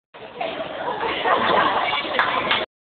Escalator Slide
this kid is a mad G you can find him at Erina Fair on the central coast of NSW Australia